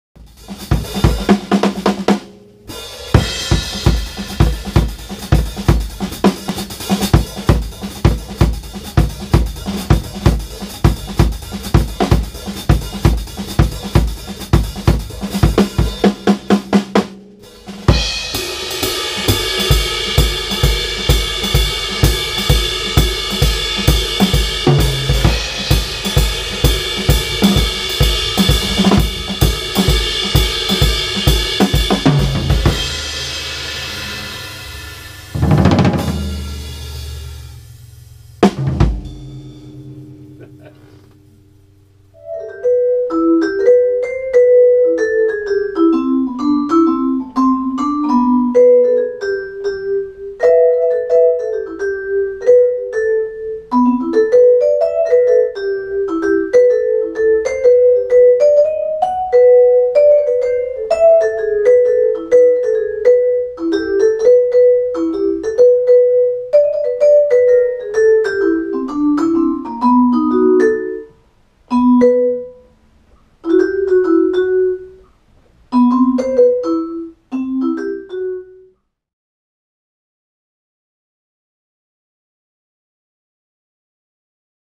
Wilkinson & Ricotti
A snoop inside the booths. Neal Wilkinson & Frank Ricotti in the session for Warner Bros. 14-4-16